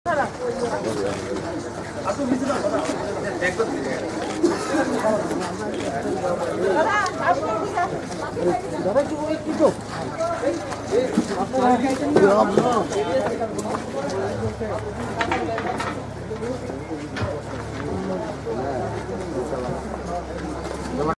Former Minister Badal Choudhury produced before Court on Monday..
TIWN 30 Dec 2019